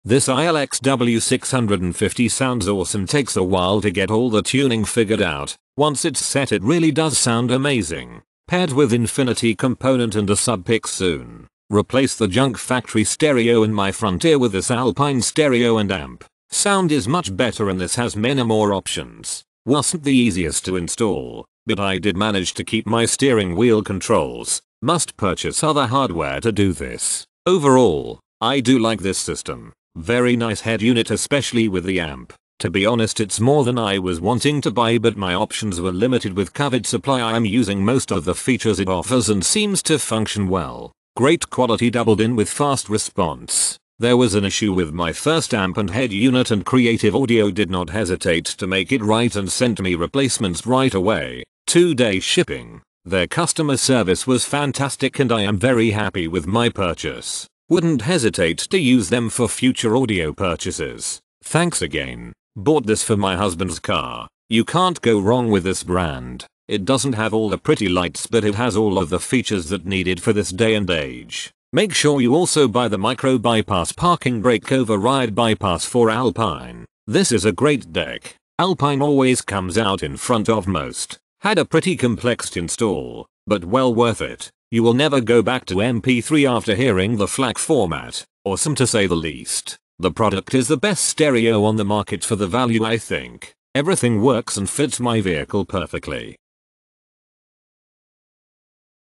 Alpine iLX-W650 Receiver Compatible with CarPlay and Android Auto - Includes review
Alpine iLX-W650 Receiver Compatible with CarPlay and Android Auto - Includes KTA-450 4-Channel Power Pack Amp

Main Features:
- shallow-mount chassis design (2-7/16" deep)
- designed to work with CarPlay and Android Auto
- The Alpine KTA-450 can mount directly to the back of the iLX-W650 for an extremely easy install.
- DPP prevents the KTA-450 from clipping by supplying up to double the power for sudden, dynamic sounds.
- Thanks to the efficient power needs of the DPP technology, the KTA-450 does not require a new power supply wire at the battery.